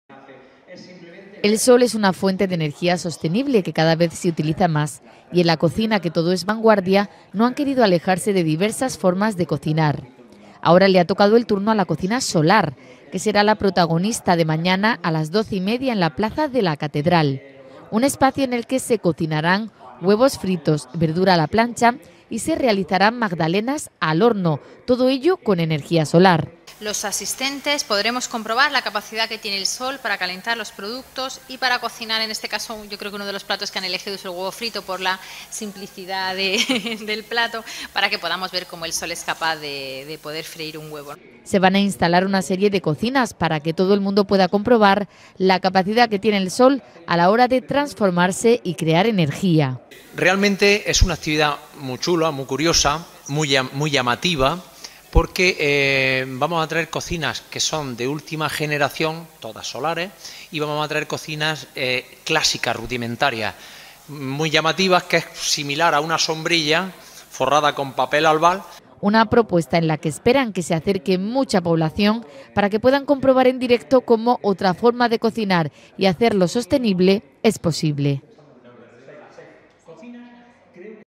La cocina solar será una de las propuestas de la Capitalidad Gastronómica de este fin de semana
Las numerosas formas de cocinar vienen dadas también por los métodos que se usen. En la sede de la capitalidad gastronómica han dado un paso más y mañana nos van a sorprender utilizando cocinas solares.